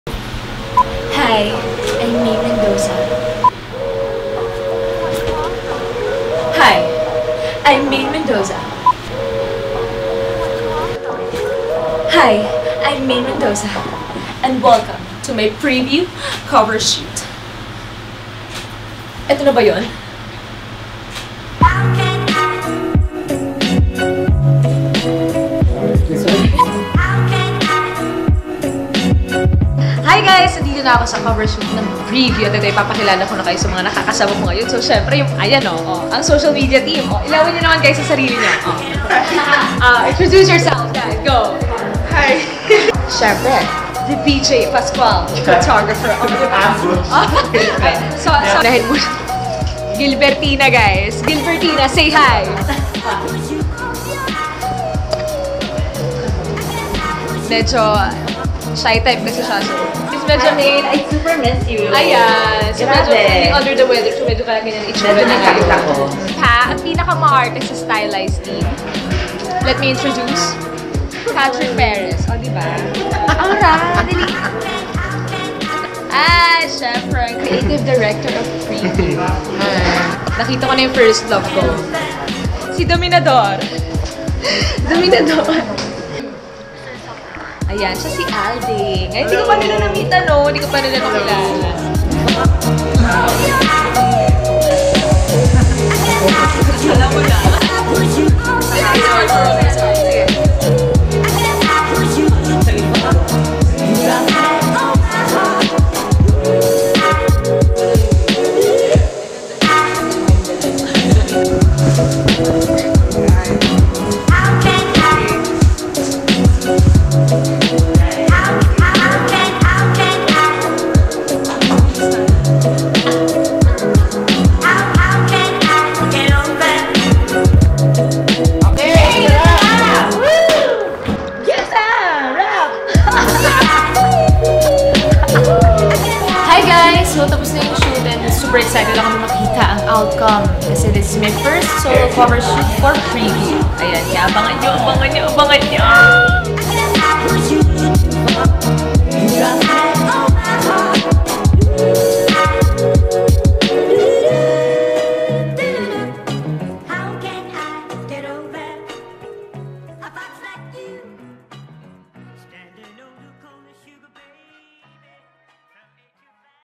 What Really Happens Behind-the-Scenes with Maine Mendoza
"I'm really shy!" Maine tells us even before our cover shoot even started, but there's no denying that this funny girl is a natural on cam. Watch the video to find out how much fun we really had on set all thanks to Maine, a selfie stick, and contagious jokes.